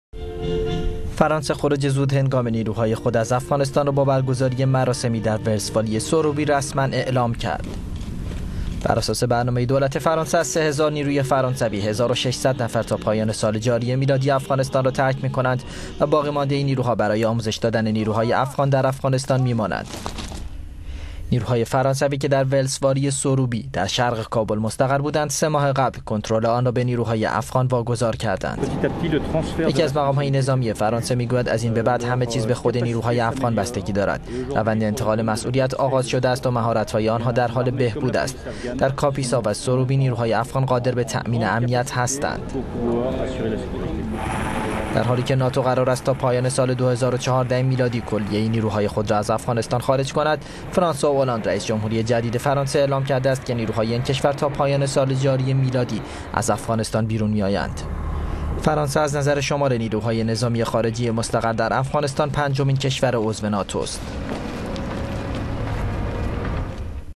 خروج نیروهای فرانسوی از افغانستان رسما آغاز شد
فرانسه خروج زودهنگام نیروهای خود از افغانستان را با برگزاری مراسمی در ولسوالی سروبی رسما اعلام کرد.

بر اساس برنامه دولت فرانسه از 3000 نیروی فرانسوی 1600 نفر تا پایان سال 2012 میلادی این کشور را ترک می کنند و باقیمانده نیروها برای آموزش دادن نیروهای افغان در افغانستان می مانند.

نیروهای فرانسوی که در ولسوالی سروبی در شرق کابل مستقر بودند، سه ماه قبل کنترل آن را به نیروهای افغان واگذار کردند. اما نیروهای این کشور از روز سه شنبه به طور رسمی خروج خود از افغانستان را آغاز کردند.

فرانسه از نظر شمار نیروهای نظامی خارجی مستقر در افغانستان پنجمین کشور عضو ناتو است. در حالی که ناتو قرار است تا پایان سال 2014 میلادی کلیه نیروهای خود را از افغانستان خارج کند، فرانسوا اولاند، رئیس جمهوری جدید فرانسه اعلام کرده است که نیروهای این کشور را تا پایان سال جاری میلادی از افغانستان بیرون می کشد. البته نیکولا سارکوزی، رئیس جمهوری پیشین هم بر خلاف هم پیمانانش در ناتو این تاریخ را پایان سال 2013 میلادی اعلام کرده بود.

پس از حمله نیروهای بین المللی به افغانستان در سال 2001 میلادی تاکنون 87 نیروی نظامی فرانسوی در افغانستان کشته شده اند.